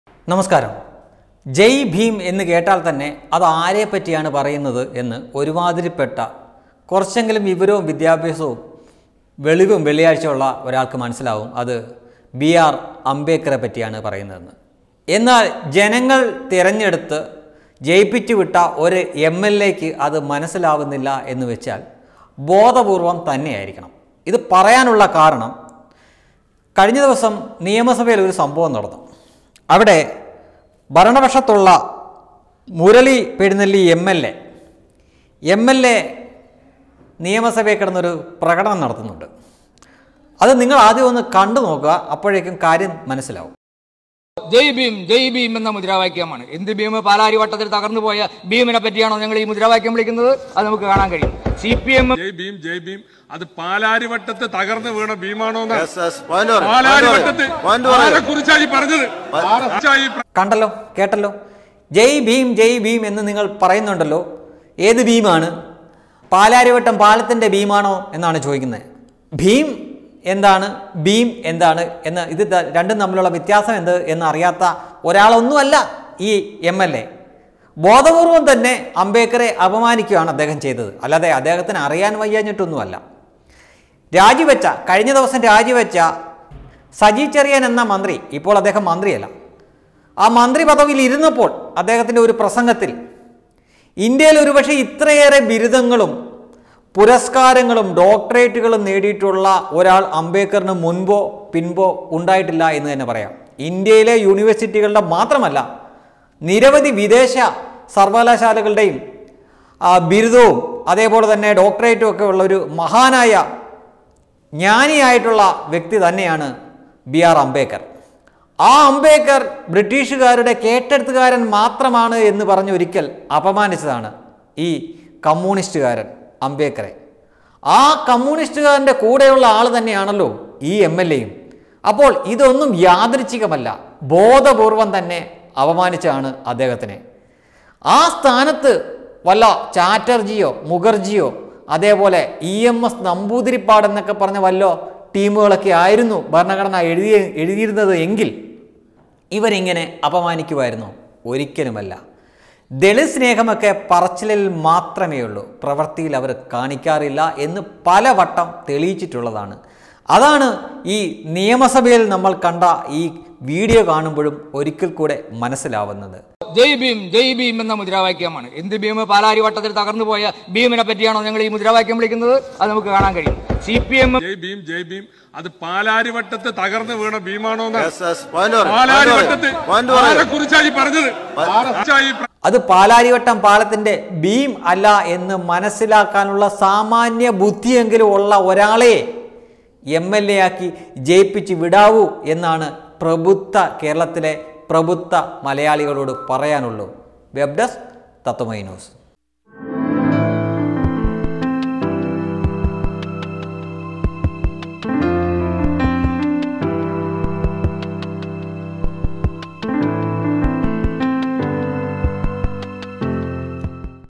സിപിഎമ്മിന്റെ ദളിത് സ്നേഹം കാപട്യം എല്ലാം വോട്ട് തട്ടാനുള്ള അടവ് | B R AMBEDKAR
അംബേദ്കറെ വീണ്ടും അപമാനിച്ച് മറ്റൊരു സിപിഎം MLA  വീഡിയോ കാണാം | MURALI PERUNELLI